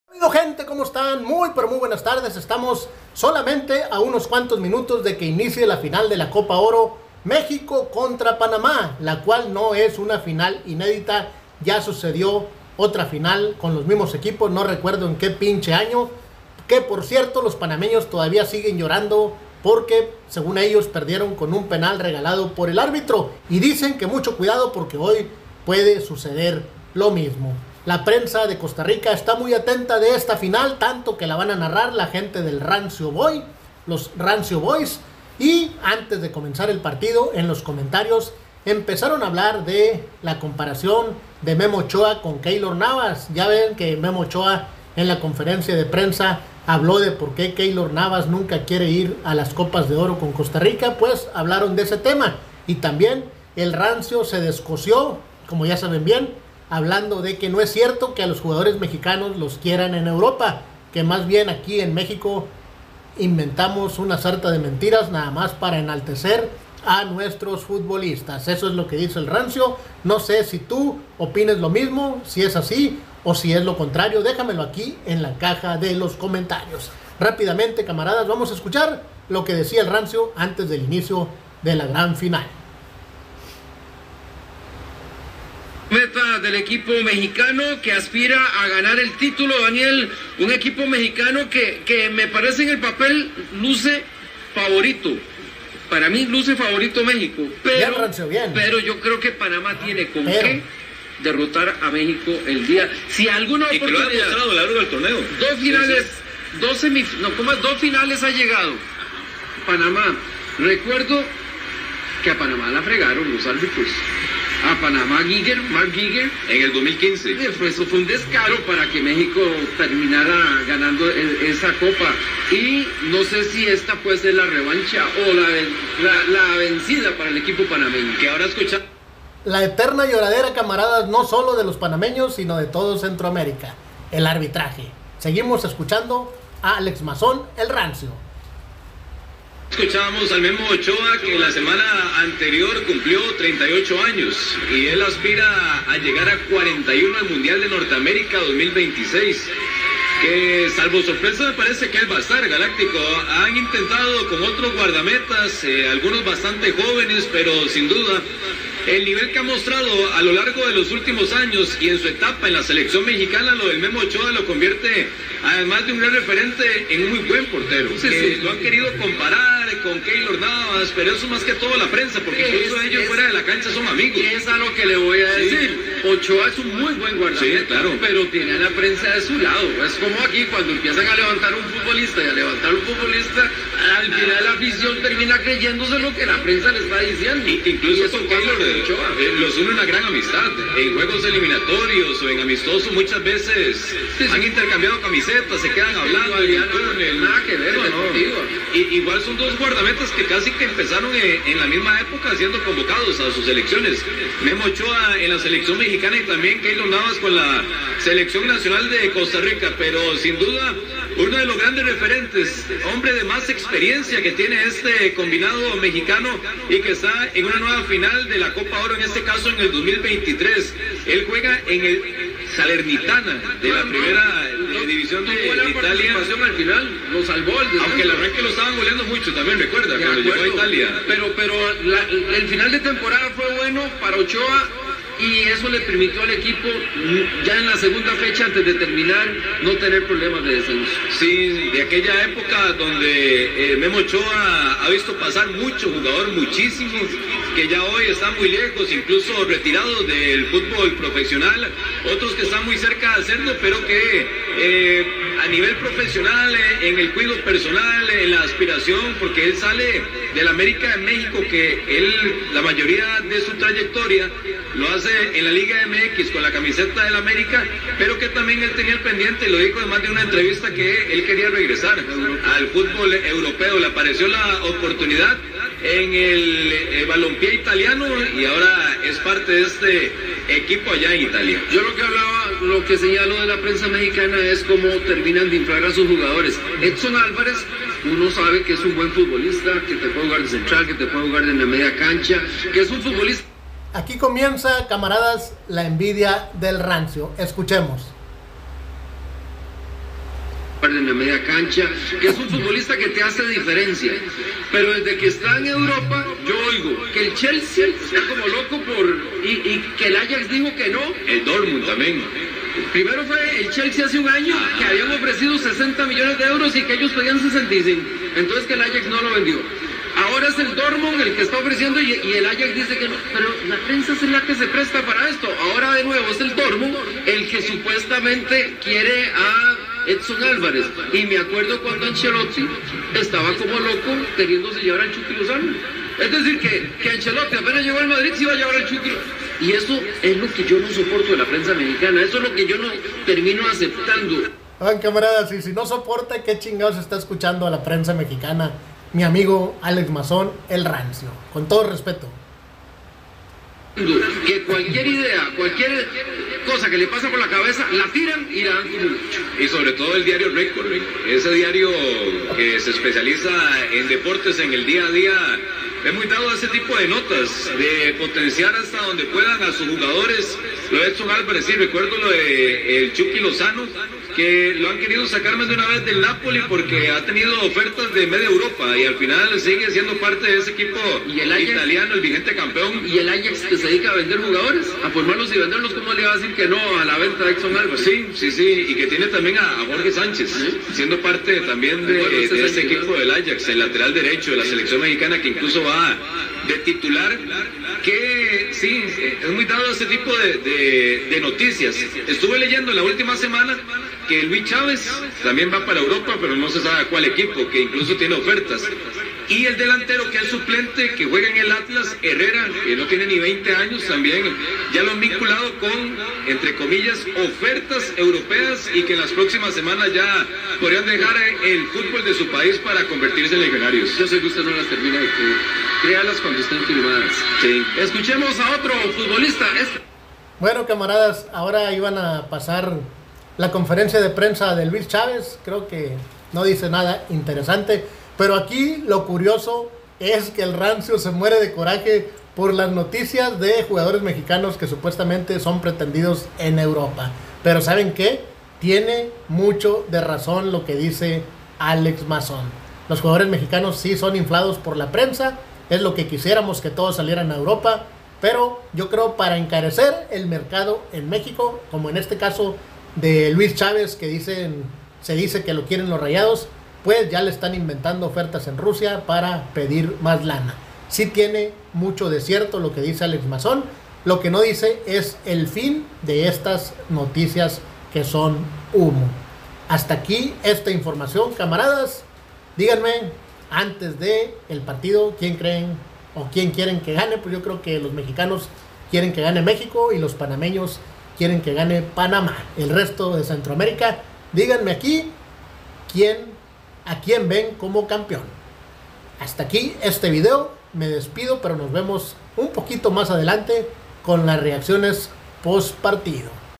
MÉXICO VS PANAMÁ EN VIVO, ESTO DIJO EL RANCIO (A LOS MEXICANOS NADIE LOS QUIERE EN EUROPA)
Prensa tica analiza ña final México vs Panamá